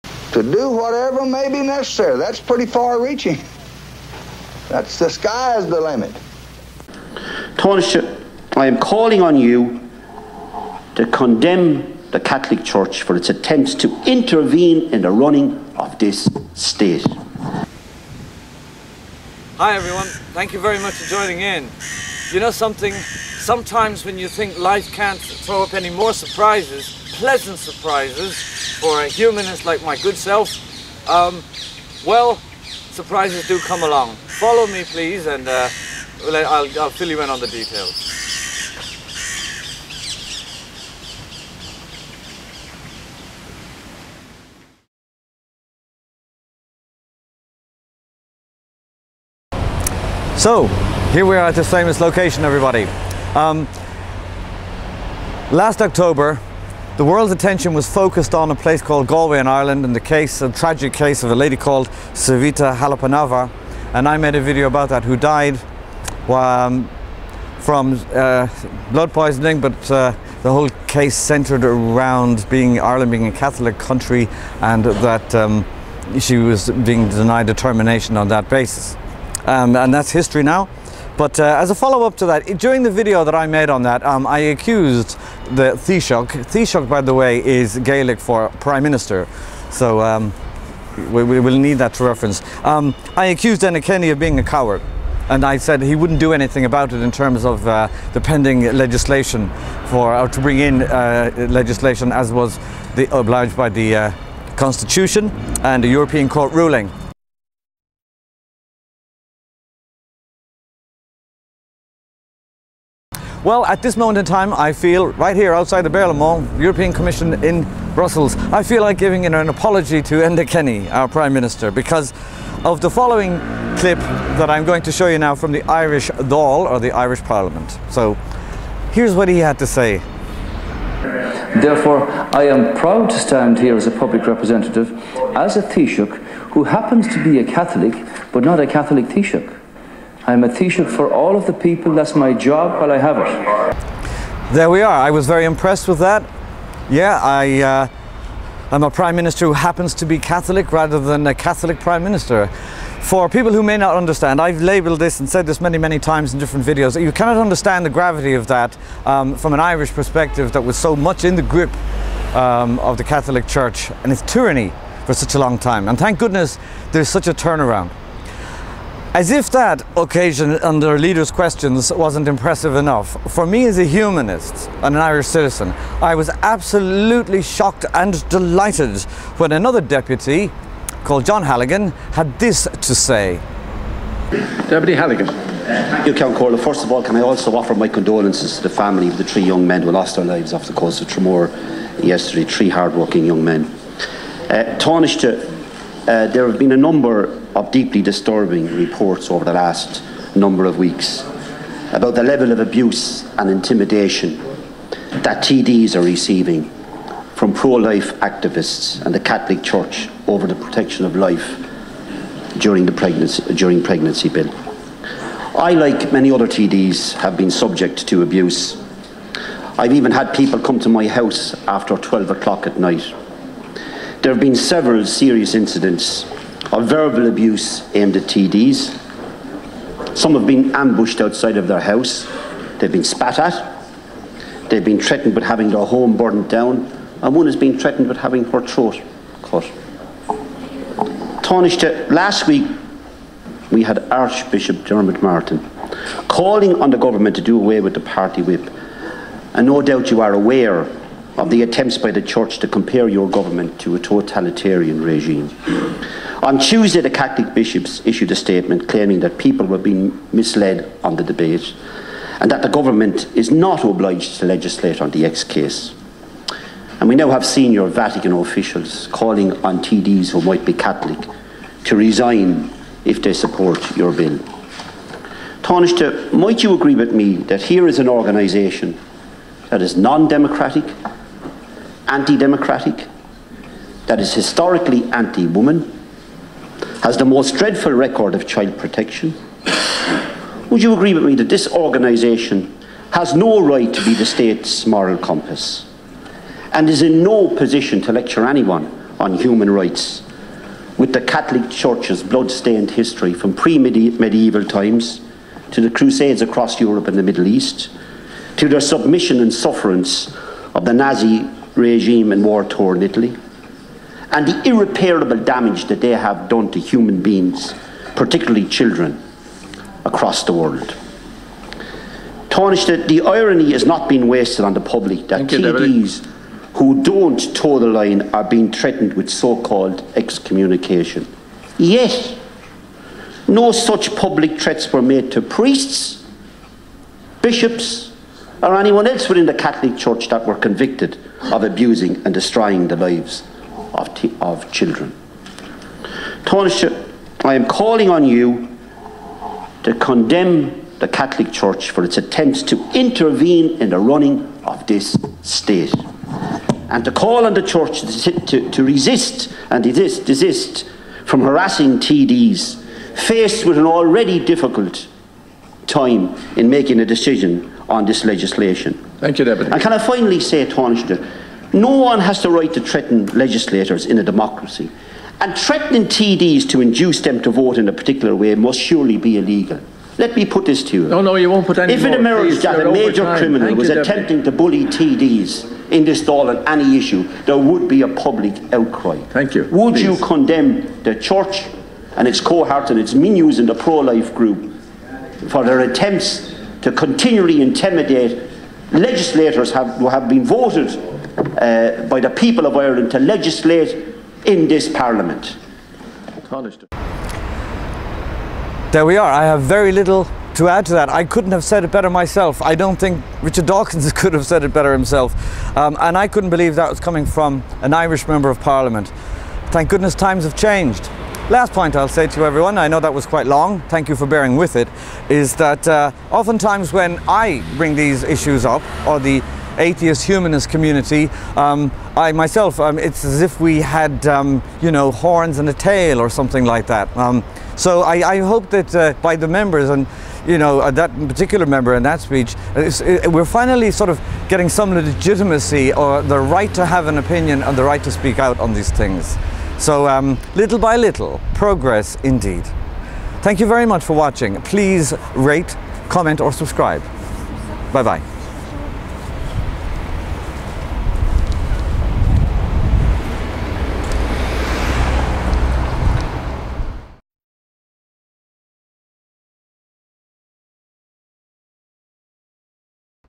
Savita, Abortion, Pregnancy Bill 2013 & Catholics!
Hopefully another triumph for women and men over the evil cult of Roman Catholic "Christianity" in Ireland. Title speaks for itself, for Savita Halappanavar.
Praveen vindicated at last.